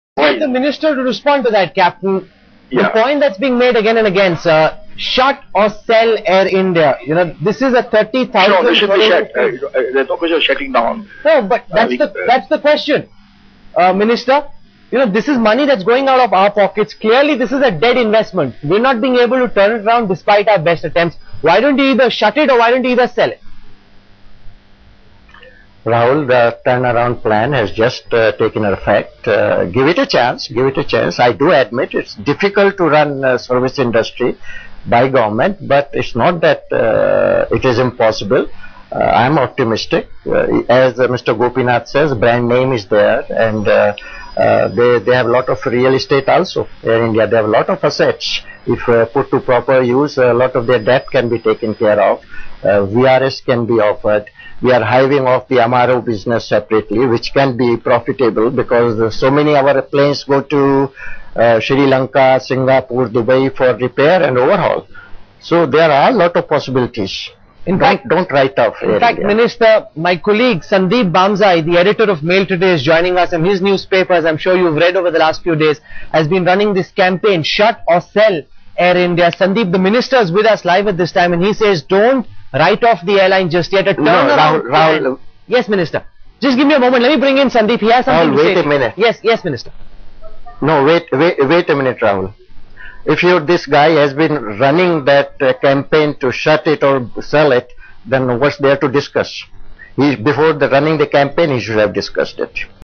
Should govt shut or sell Air India?-6
Pilots strike costs government over Rs 300 crore.